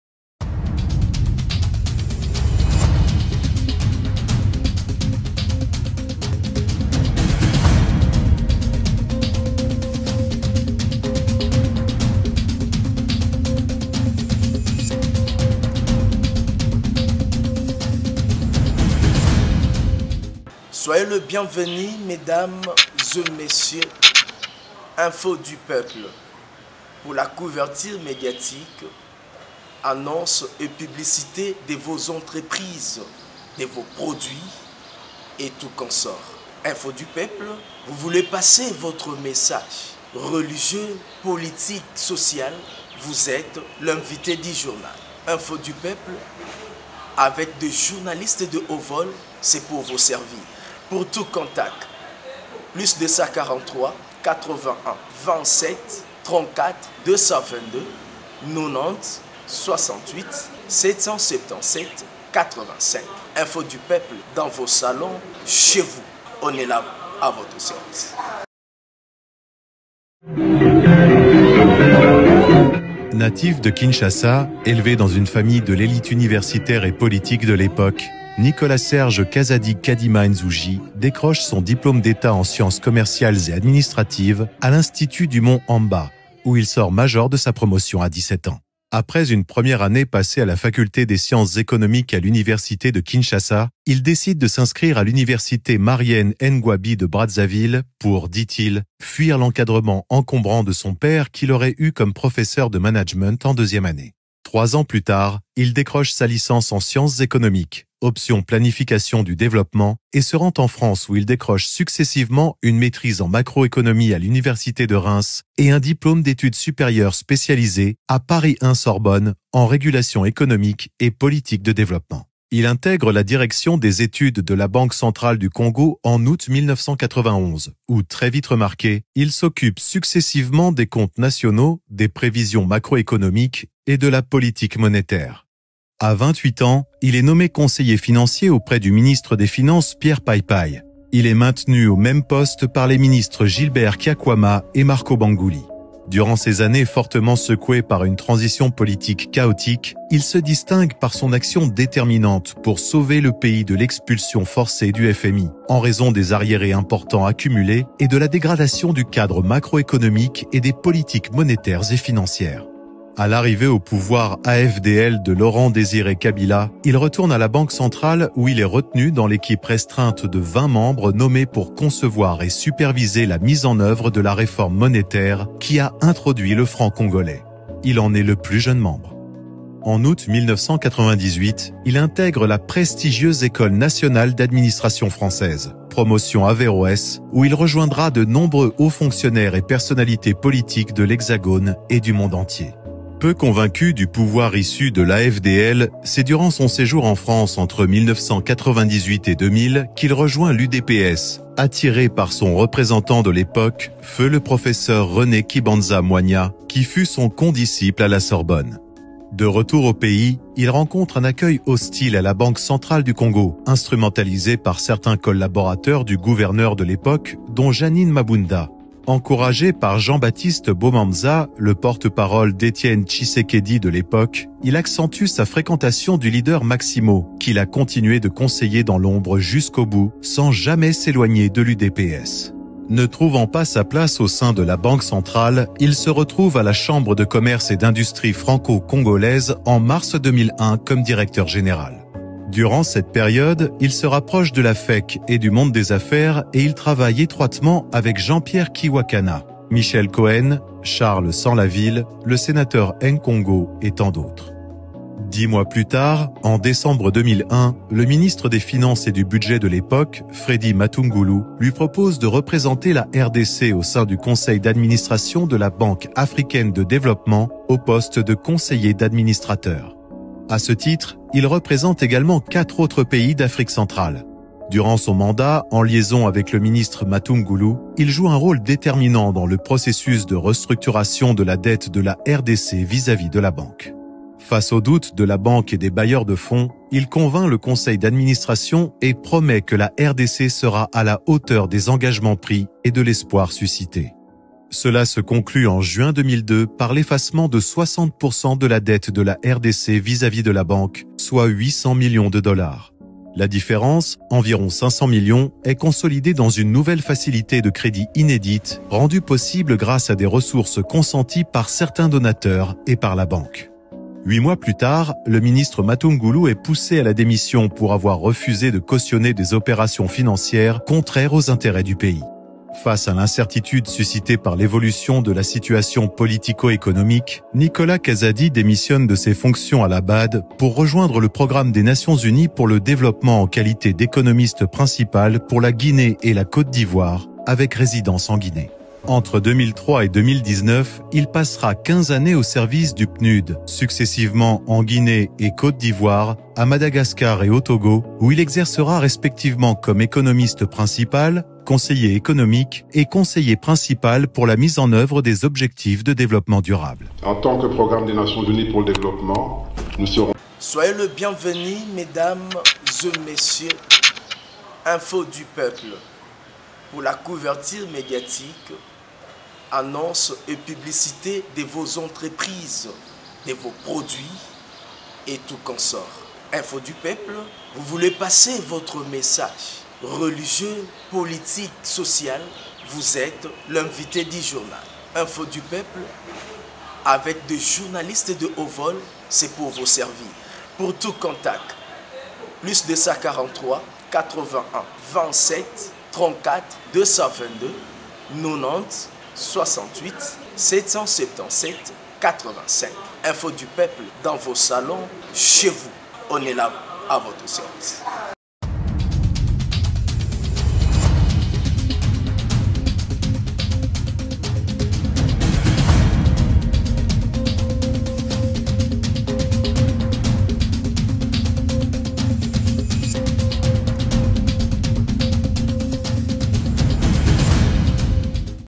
CE QU'IL FAUT RETENIR SUR NICOLA KAZADI LE NOUVEAU MINISTRE DE FINANCE
Infos du peuple